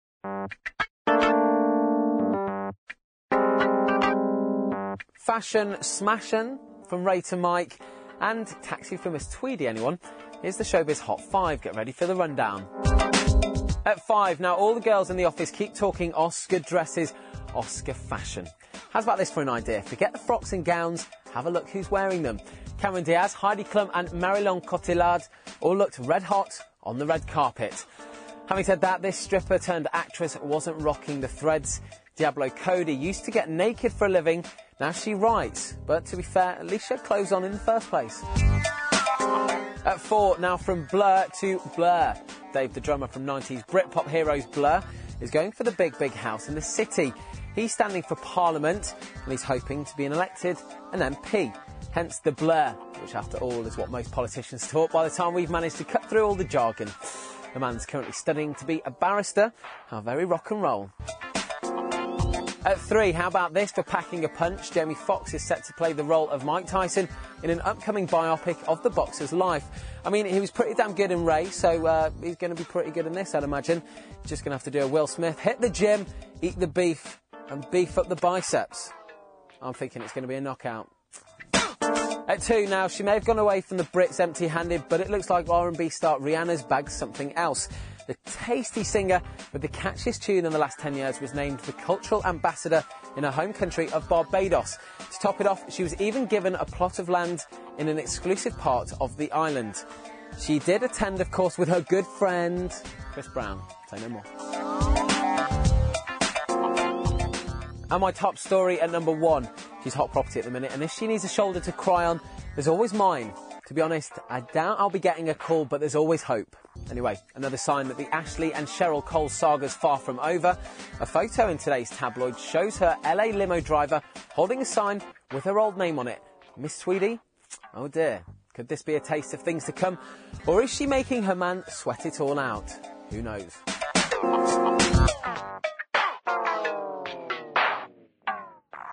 Hot babes on the Oscar red carpet
Hot babes on the Oscar red carpet - Cheryl uses Tweedy as surname - Jamie Foxx to play Mike Tyson. Test